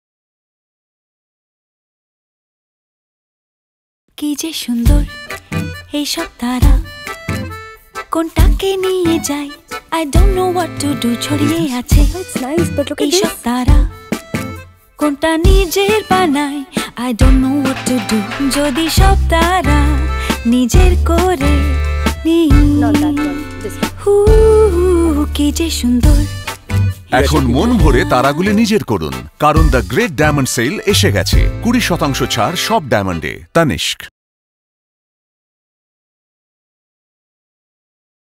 Paroma Dasgupta |Tanishq jewellery Ad | Bengali
Loved singing in multiple languages for this ad :) here’s the Bengali one. Enjoy !!